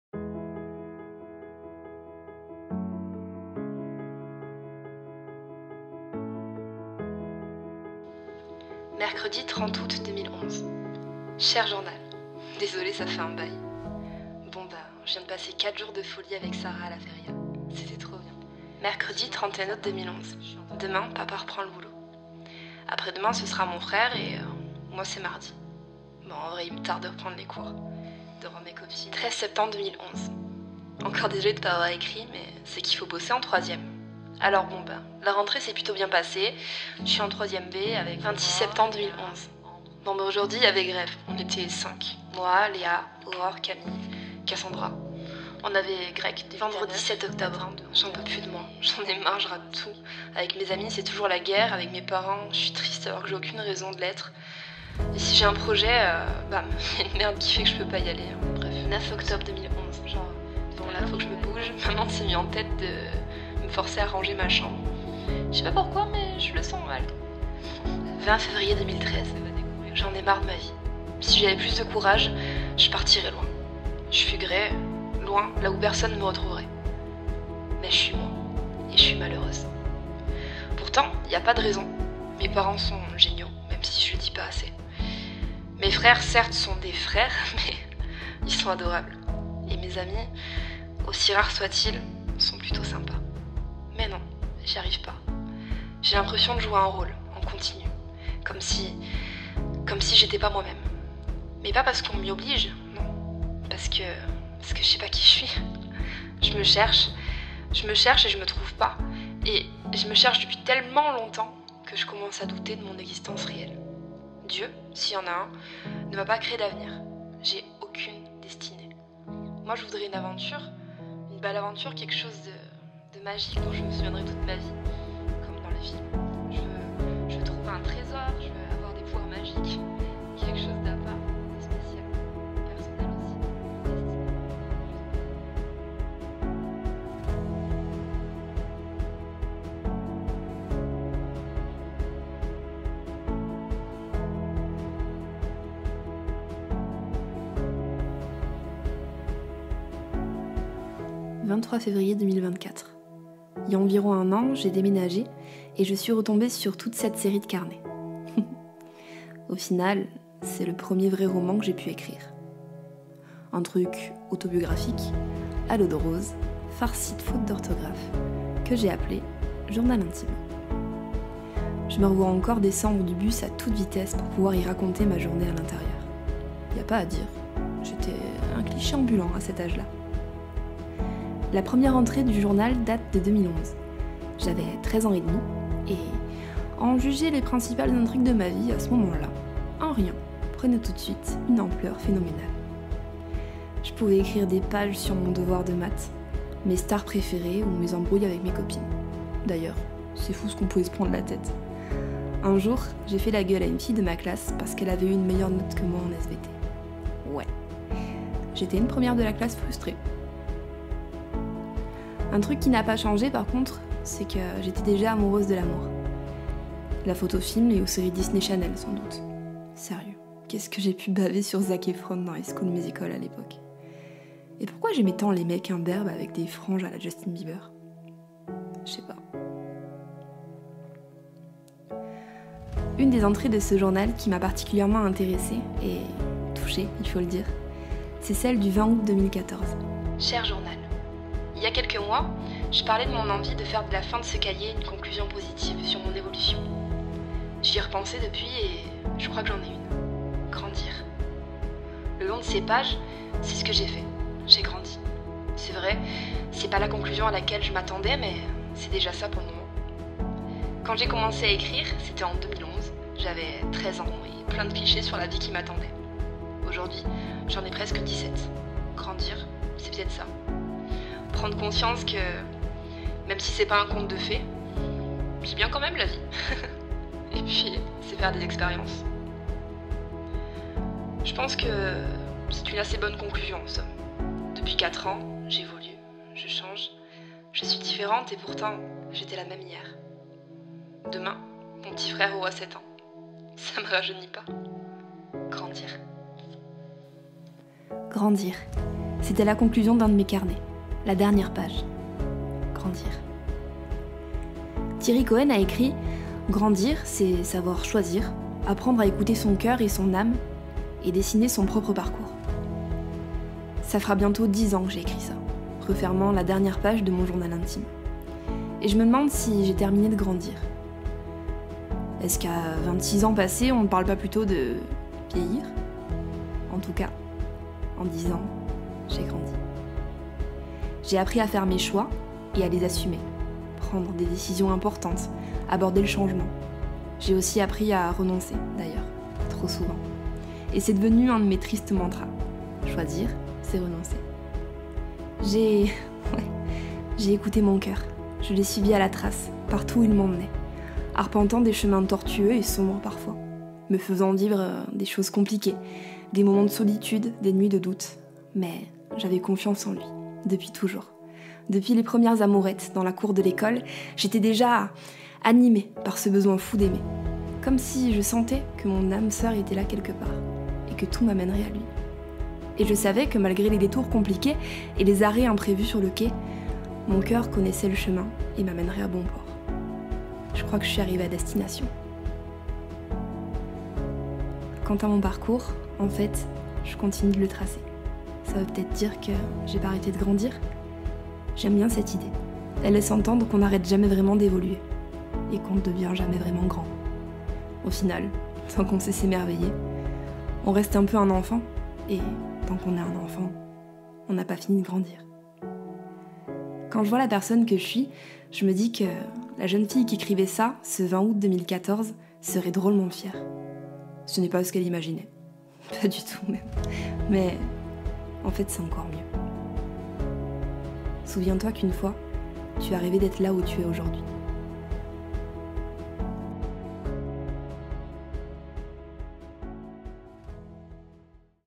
Cher Journal
Il y a quelques temps, je suis retombée sur mes vieux carnets. Finalement, le premier roman que j'ai pu écrire, c'était ça : mon journal intime.
Alors... presque dix ans après l'avoir terminé... j'ai décidé d'écrire une conclusion. La voilà.